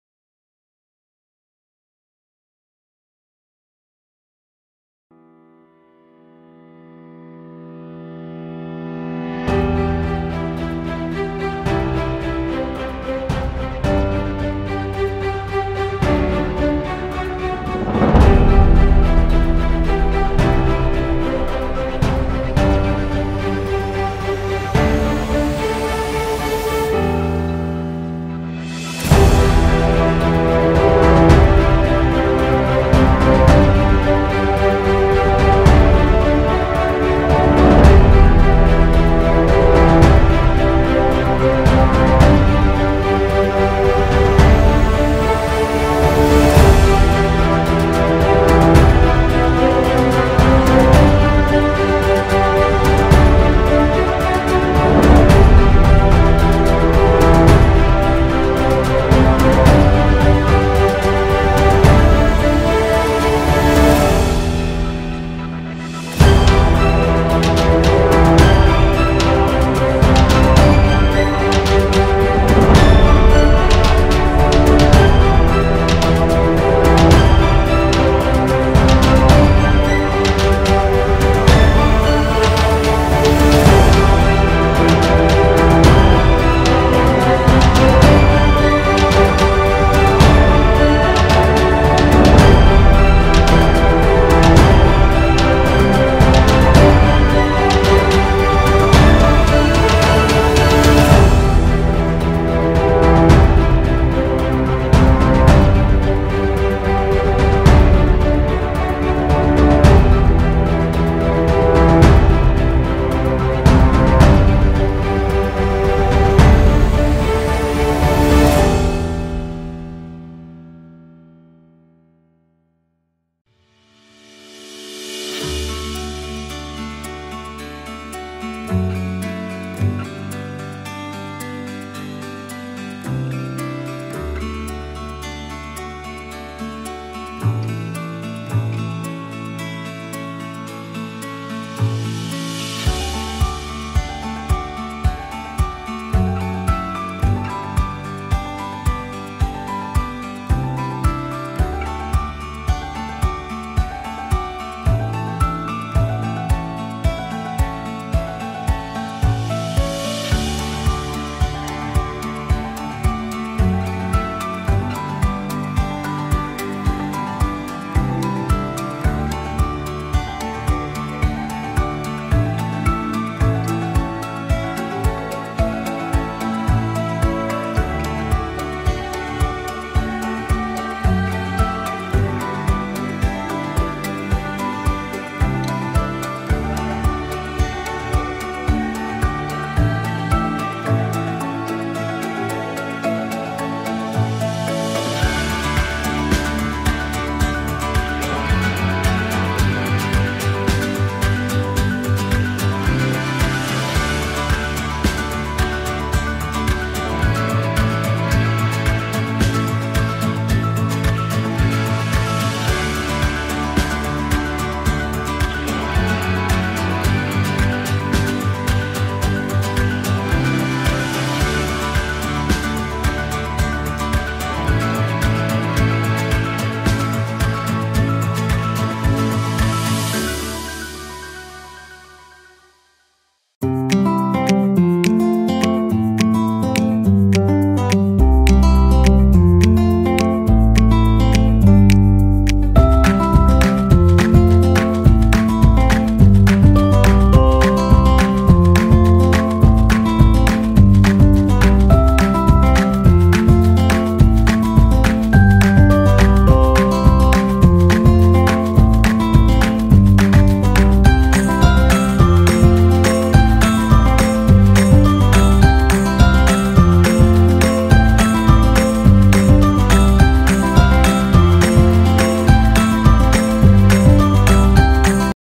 Sublimating on Canvas
Sublimation on canvas

This is the laminating film I used

Heat press temp 385 degrees Fahrenheit for 50 seconds